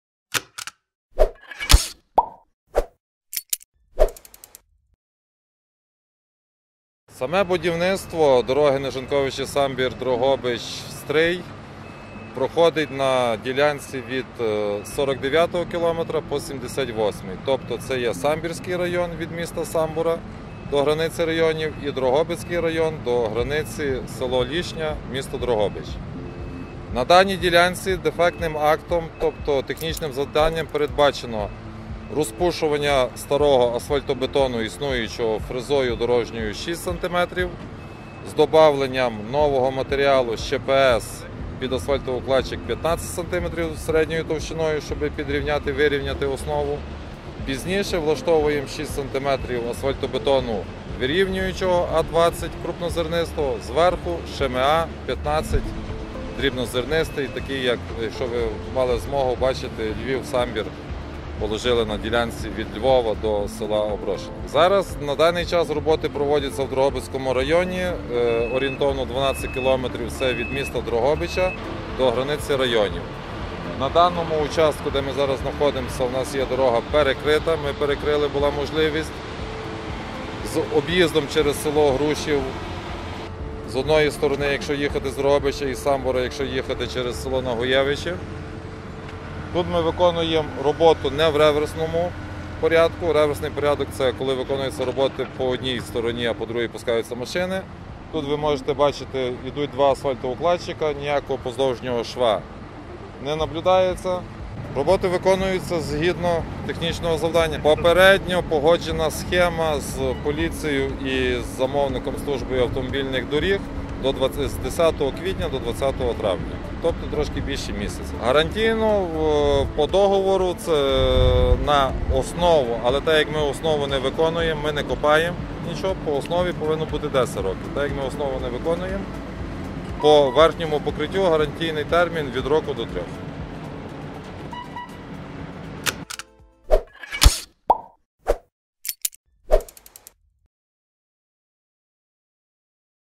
Нова дорога “Дрогобич - Самбір”. Роботи тривають …
Вже до початку літа між Дрогобичем та Самбором буде відремонтовано дорогу, яку цієї зими назвали найгіршою в Україні.

У квітні на цьому відрізку дороги розпочався ремонт, який здійснює компанія “ОНУР”. Після завершення робіт тут буде облаштовано нове дорожнє полотно, 24 пішохідних переходи та близько 500 дорожніх знаків.

Наш репортаж з дороги “Дрогобич - Самбір”.
Відео - Варто Галицькі Новини (VARTO_TV)
Тарас Іванишин
Олег Дьорка